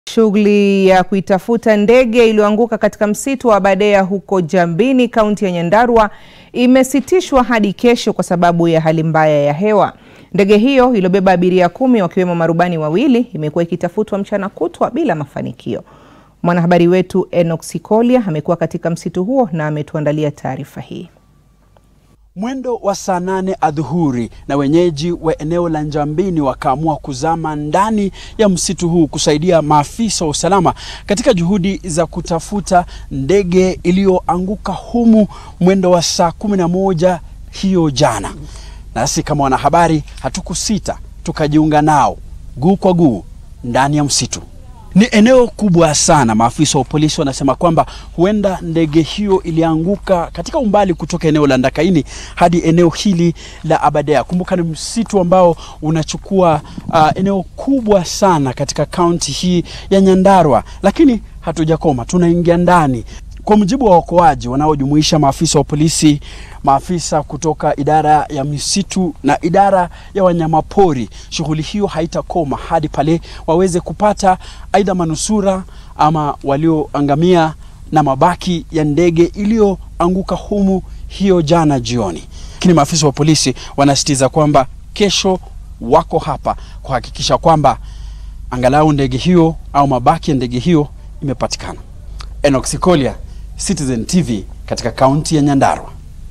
Shughuli za kutafuta ndege iliyoanguka zasitishwa
Shughuli ya kuitafuta ndege iliyoanguka katika msitu wa Aberdare huko Njabini kaunti ya Nyandarua imesitishwa hadi kesho kwa sababu ya hali mbaya ya hewa. Ndege hiyo iliyobeba abiria kumi wakiwemo marubani wawili imekuwa ikitafutwa mchana kutwa bila ya mafanikio. Mwanahabari wetu enock sikolia amekuwa katika msitu huo na ameandaa taarifa ifuatayo